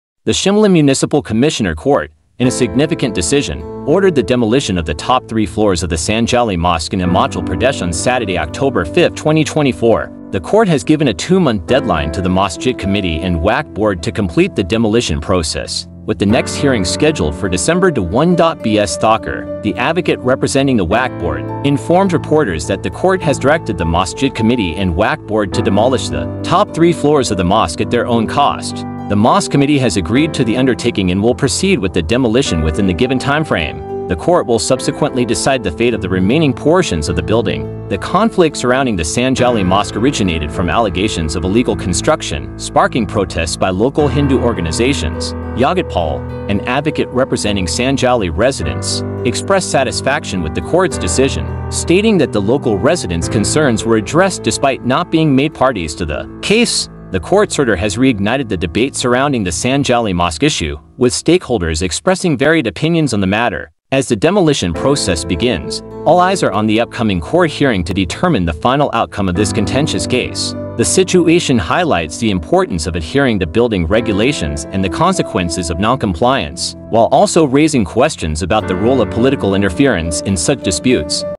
URGENT: Shimla Court Orders Mosque Demolition
The Shimla Municipal Commissioner Court, in a significant decision, ordered the demolition of the top three floors of the Sanjauli Mosque in Himachal Pradesh on Saturday, October 5, 2024. The court has given a two-month deadline to the Masjid committee and Waqf Board to complete the demolition process, with the next hearing scheduled for December 21.

BS Thakur, the advocate representing the Waqf Board, informed reporters that the court has directed the Masjid committee and Waqf Board to demolish the top three floors of the mosque at their own cost. The mosque committee has agreed to the undertaking and will proceed with the demolition within the given timeframe. The court will subsequently decide the fate of the remaining portions of the building.

The conflict surrounding the Sanjauli Mosque originated from allegations of illegal construction, sparking protests by local Hindu organizations. Jagat Paul, an advocate representing Sanjauli residents, expressed satisfaction with the court's decision, stating that the local residents' concerns were addressed despite not being made parties to the case.

The Sanjauli Mosque issue resurfaced following a visit by AIMIM Delhi Chief Shoaib Jamai, who posted a video on social media alleging discriminatory treatment of the mosque compared to other illegal constructions in the area. However, the Sanjauli Mosque Committee and the local Muslim community distanced themselves from Jamai's remarks, condemning his statement as detrimental to Shimla's peaceful environment and calling for appropriate action against him.

The court's order has reignited the debate surrounding the Sanjauli Mosque issue, with stakeholders expressing varied opinions on the matter. As the demolition process begins, all eyes are on the upcoming court hearing to determine the final outcome of this contentious case. The situation highlights the importance of adhering to building regulations and the consequences of non-compliance, while also raising questions about the role of political interference in such disputes."

💬 Comment your thoughts on the court's decision! 📢 Stay informed, stay engaged! 🌟 Thank you for your support!